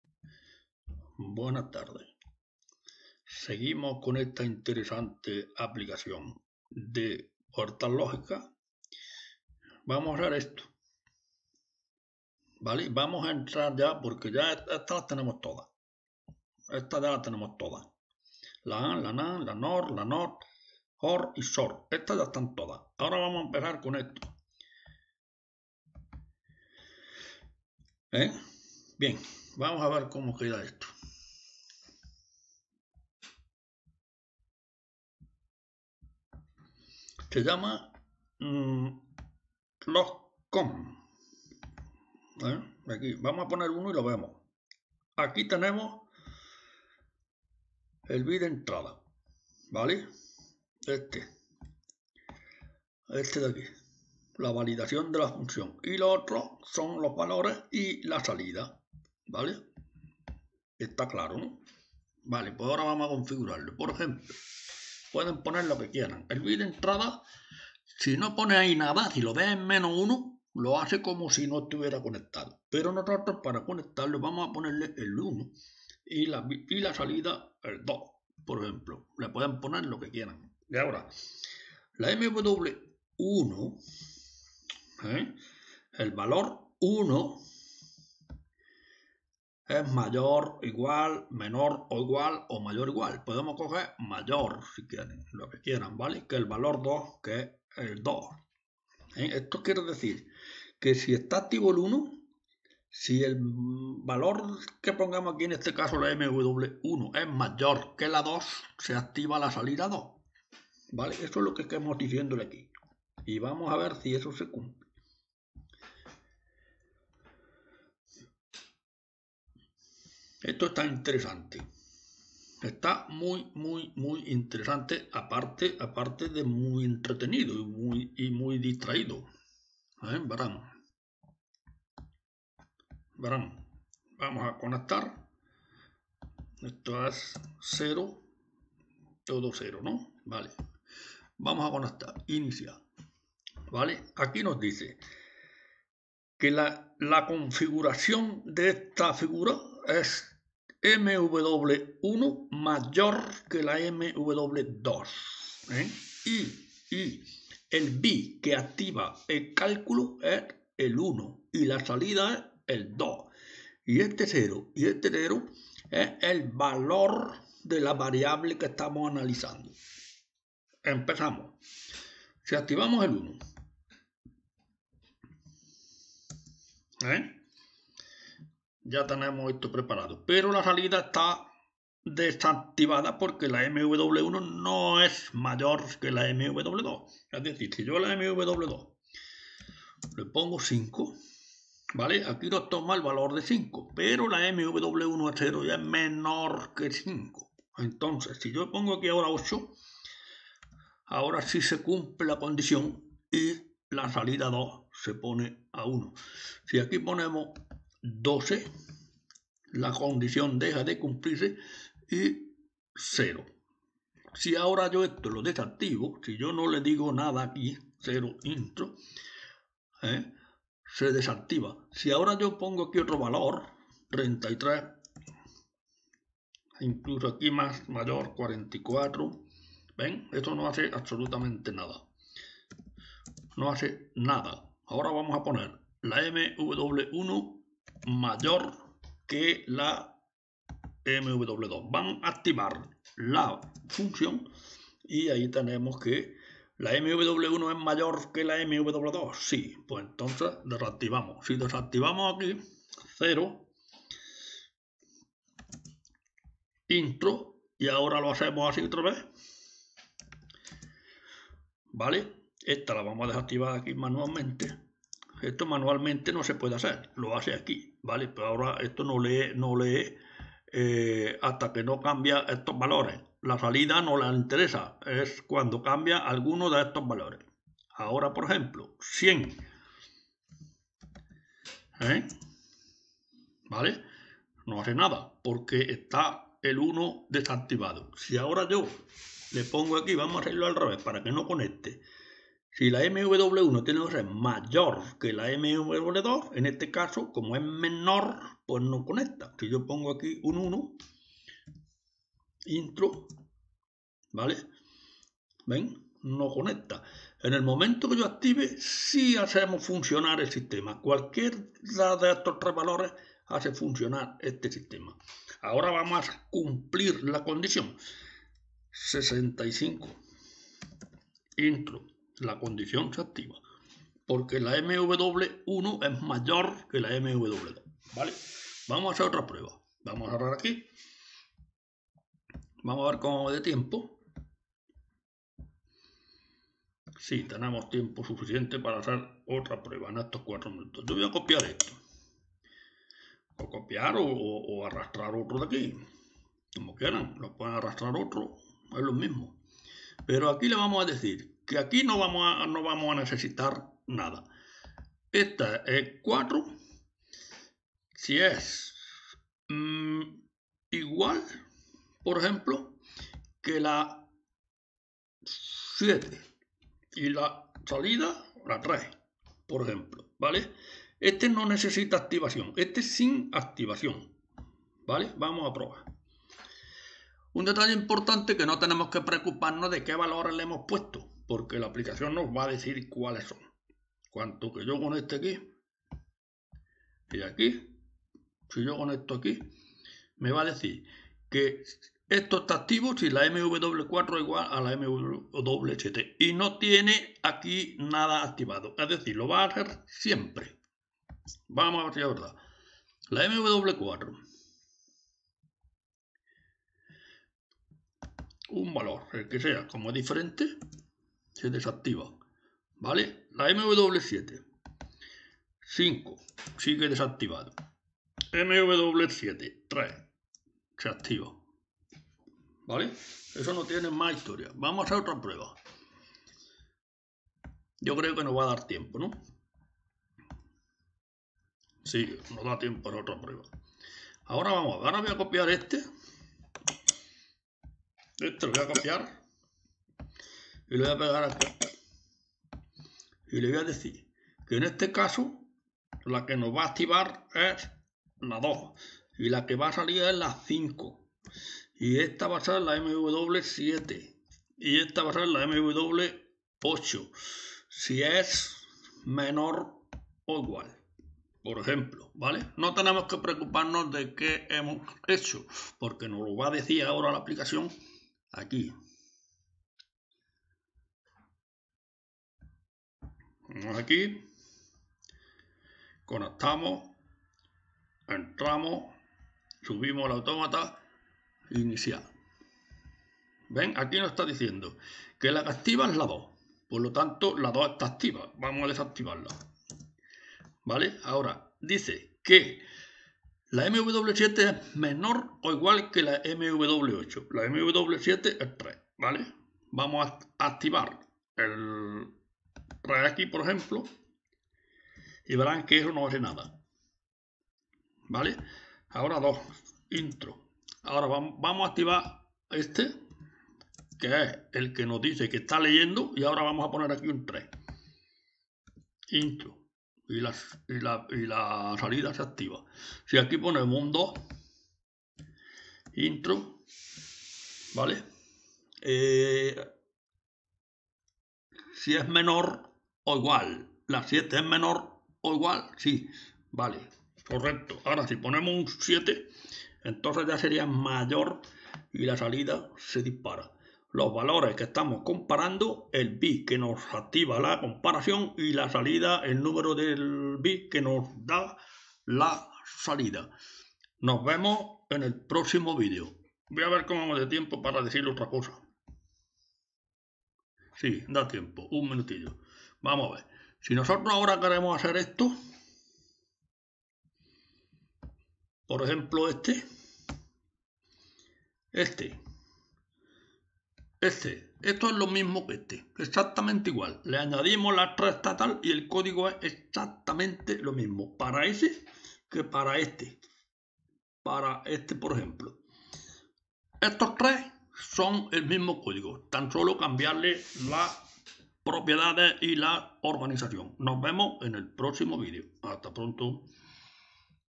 PUERTAS LOGICAS + BDF EN PLC P4 (COMPARACION)
Comparación lógica realizada, por supuesto con todo lujo de detalles para que la configuración o las pruebas sean lo más legibles posible de un vistazo.

Una vez creado esta componente de usuario, los demás caen por su peso, ya que lo único que los difiere es el código del evento, próxima parada multiplexado, es decir. seleccionar entre dos valores mediante un bit, lo vemos en el próximo vídeo...